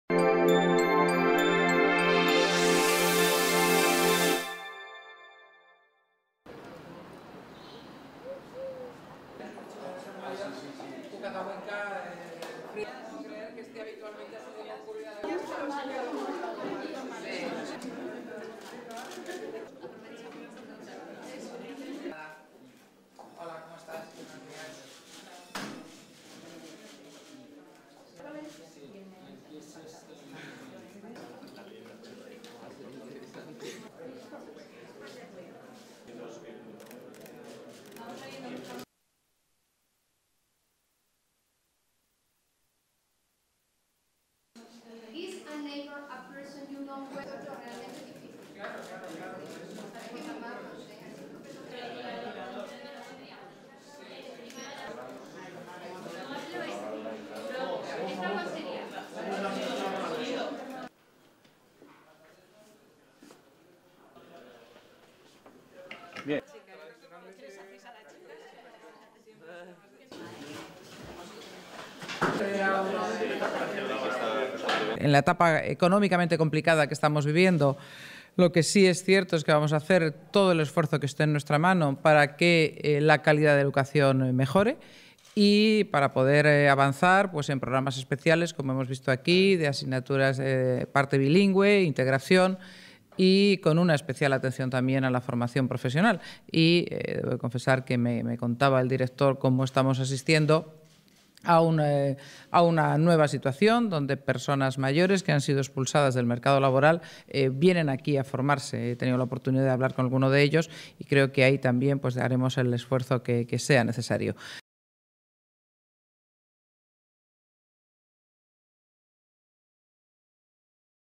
Apertura del curso escolar 2011 - 2012
La Presidenta del Gobierno de Aragón, Luisa Fernanda Rudi, acompañada por la consejera de Educación, Universidad, Cultura y Deporte, Dolores Serrat, ha visitado el Instituto de Educación Secundaria "Tiempos Modernos" de Zaragoza. Rudi ha conocido las instalaciones del centro y ha saludado a profesores y alumnos. La Presidenta ha recorrido algunas aulas de Educación Secundaria Obligatoria, Bachillerato y Formación Profesional.

Durante su visita, Luisa Fernanda Rudi ha explicado que "a pesar de vivir una etapa económicamente complicada, el Ejecutivo hará el máximo esfuerzo para que la calidad de la educación mejore y para poder avanzar en programas especiales como el de bilingüismo o la integración". Además, ha dicho, "prestará una especial atención a la formación profesional".

La Presidenta de Aragón ha destacado que el Gobierno hará un esfuerzo para afrontar una nueva situación a la que asiste la sociedad. Rudi ha recordado que "en la actualidad algunas personas adultas, excluidas del mundo laboral, regresan a los centros educativos para formarse". La Presidenta ha asegurado que el Ejecutivo autonómico "no va a recortar en materia educativa sino que buscará la mayor eficiencia y eficacia de los recursos disponibles".

A este curso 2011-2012 se han incorporado a las aulas cerca de 210.000 alumnos, unos 900 más que el curso pasado. La cifra de alumnos ha aumentado significativamente en Infantil y Formación Profesional.